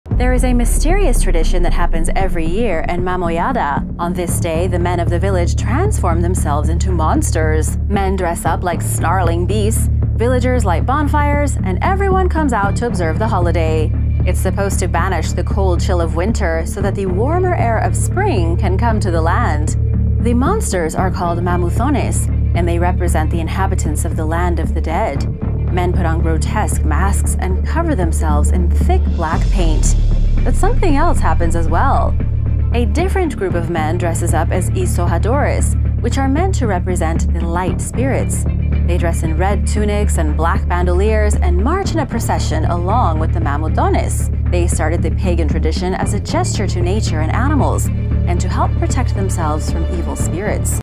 The Mysterious Tradition In Sardinia Italy!
There is a mysterious tradition that happens every year in Mamoiada, a small village in the countryside of Sardinia. On this day, the men of the village transform themselves into monsters.
 Men dress up like snarling beasts, villagers light bonfires, and everyone comes out to observe the holiday. It's supposed to banish the cold chill of winter so that the warmer air of spring can come to the land. It’s been going on for over 2000 years, since before Christianity.